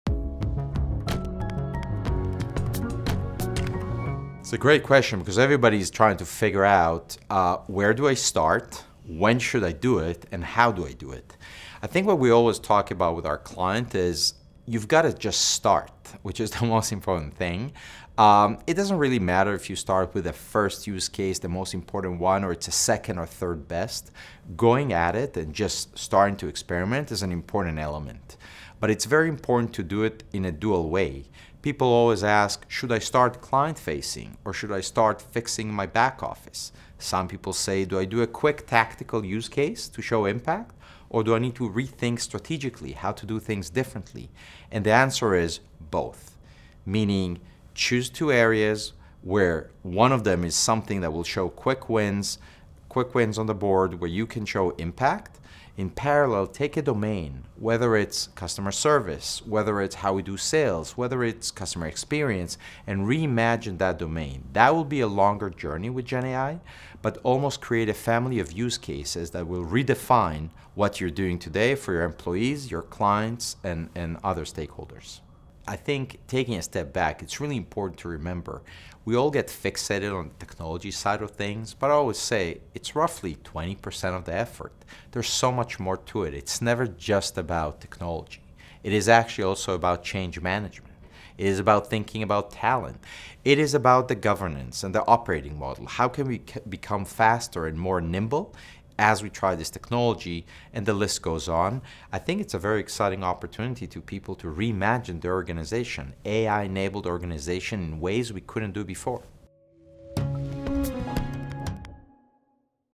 How can QuantumBlack guide C suite investment in gen AI?
Ben Ellencweig, a McKinsey Senior Partner and Global Leader of alliances and partnerships for QuantumBlack, AI by McKinsey, explains how the c-suite can invest in gen AI.